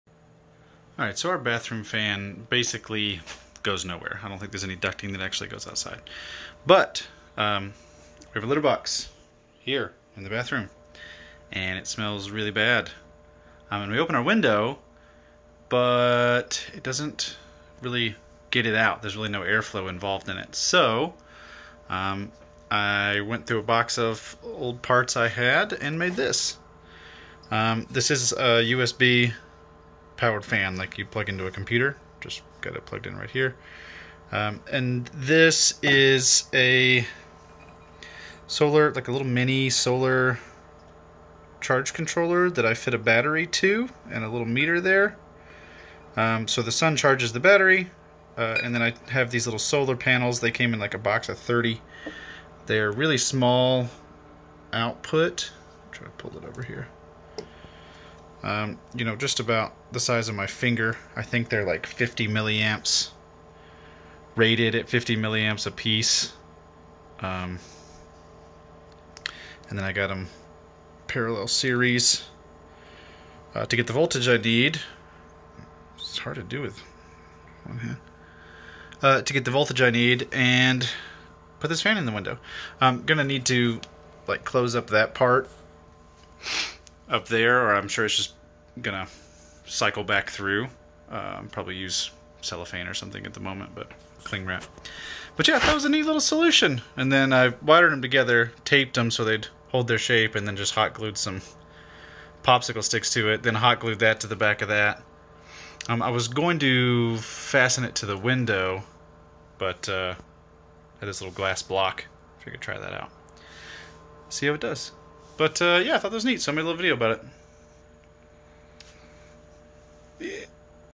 Small Solar Exhaust Fan
Our bathroom needed an active way to vent out the window so I made this from spare parts.